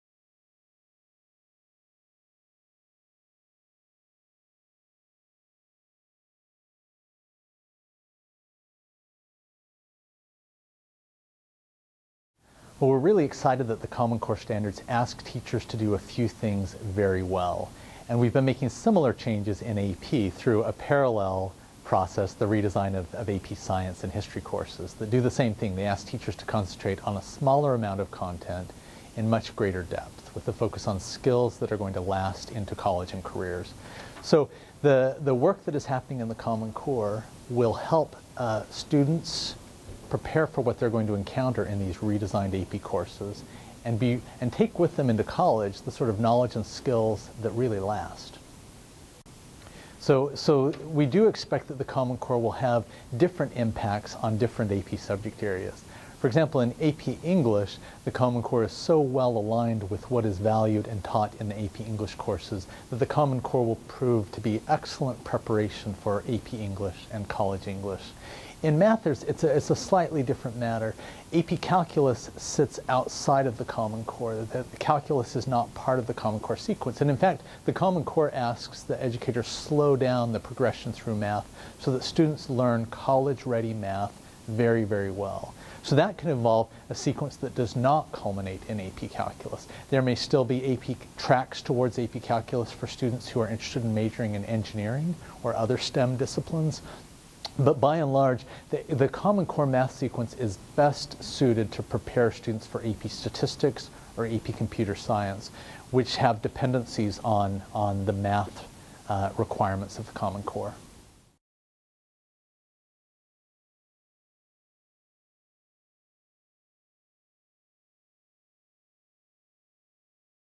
College Board's Trevor Packer on Common Core and AP Curriculum
At AASA's 2013 National Conference on Education, Trevor Packer, senior vice president of College Board, discusses how the Common Core standards will influence Advanced Placement curriculum in English and mathematics.